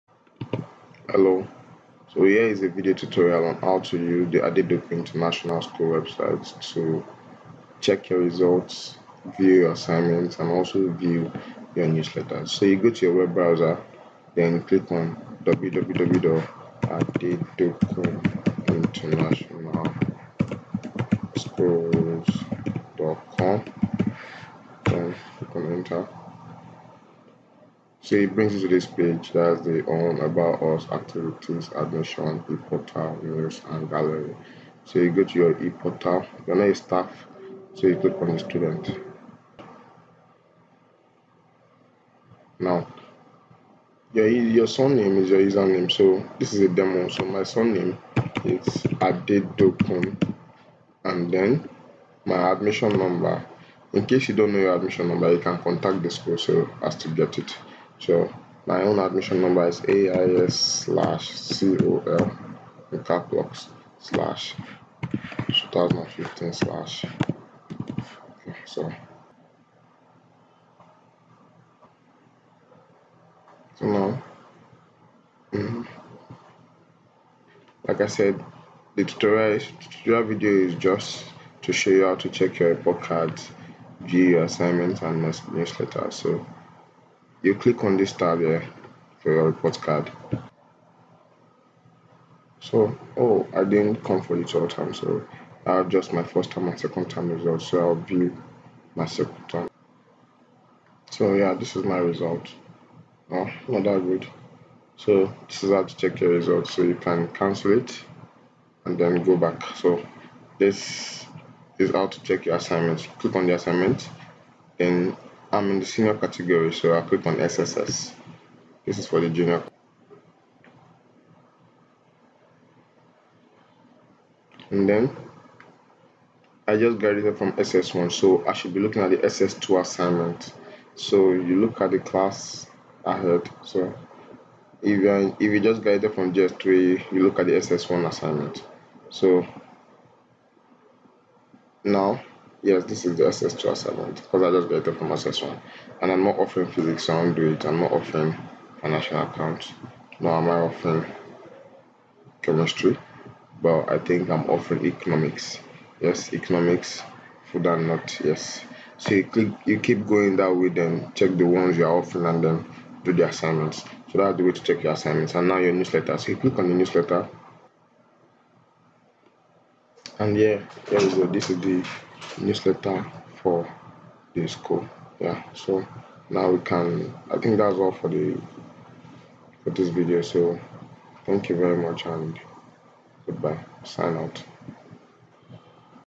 AIS Video Tutorial
How to check results, view assignments and look through newsletters